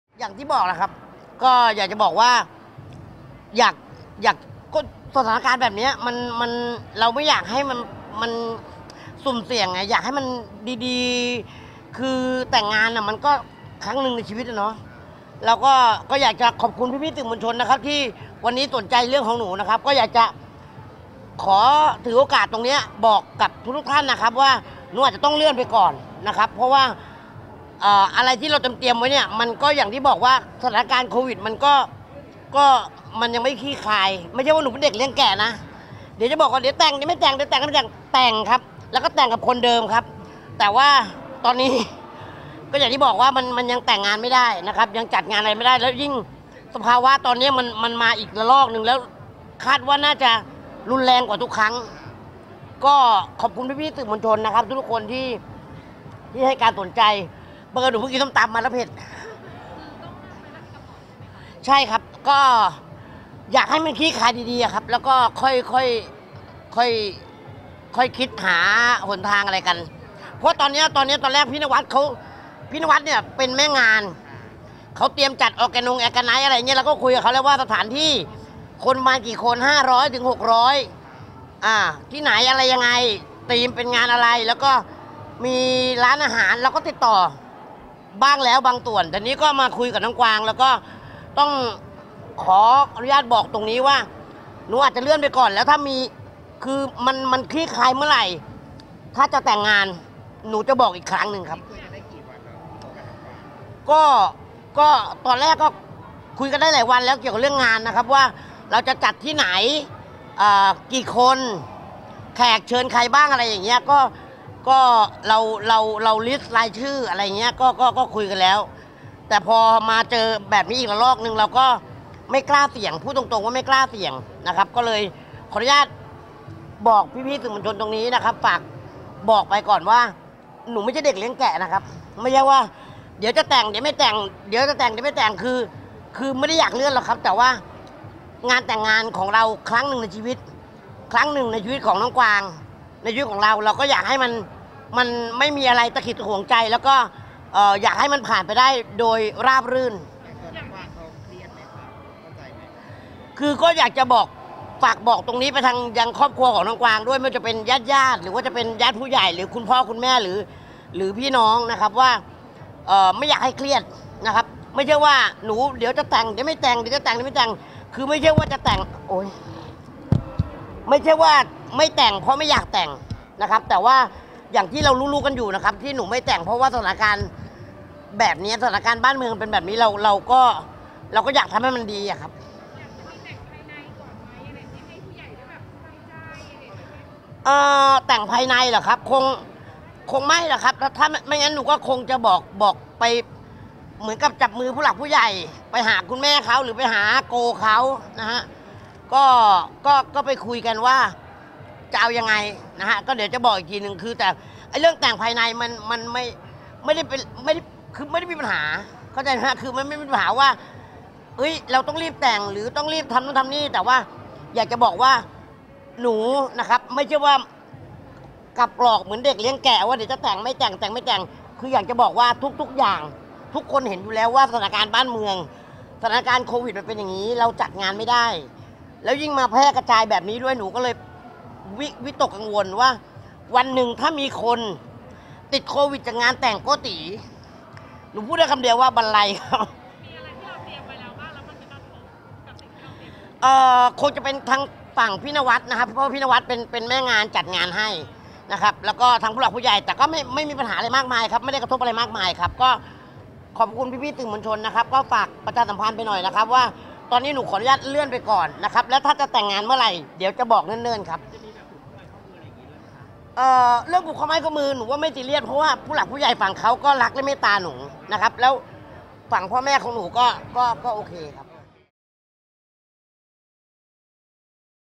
โก๊ะตี๋ ไม่กล้าเสี่ยง!! ประกาศขอเลื่อนงานแต่งออกไปก่อน
โก๊ะตี๋อารามบอย ไม่กล้าเสี่ยง!! ประกาศขอเลื่อนงานแต่งออกไปก่อน ยันแต่งน้องกวางแน่ แต่ยังไม่สามารถระบุวันได้ โอดตอนนี้สถานการณ์ไม่ปกติ คาดโควิดระลอกนี้แรงกว่าที่ผ่านมา ยันฝ่ายหญิงและผู้ใหญ่เข้าใจ